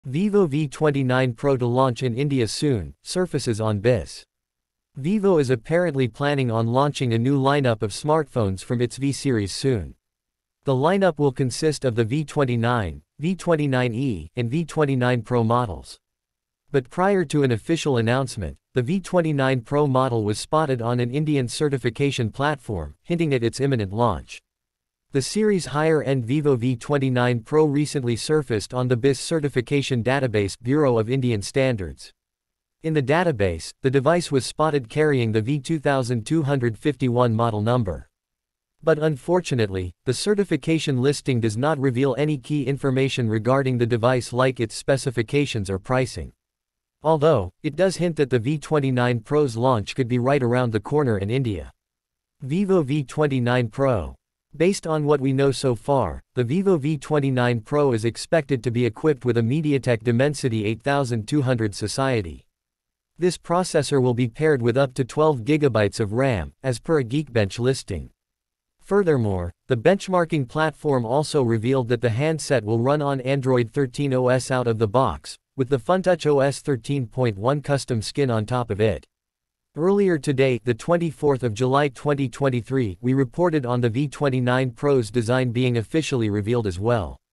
Vivo V29 5G specs & European pricing leaked ahead of global launch
Vivo will soon be launching the Vivo V29 series smartphone in multiple markets. The series is expected to have a vanilla V29 and a V29 Pro smartphone. Ahead of the launch, a new report from Appuals has revealed the full specs sheet of the Vivo V29 5G along with its price in the Czech Republic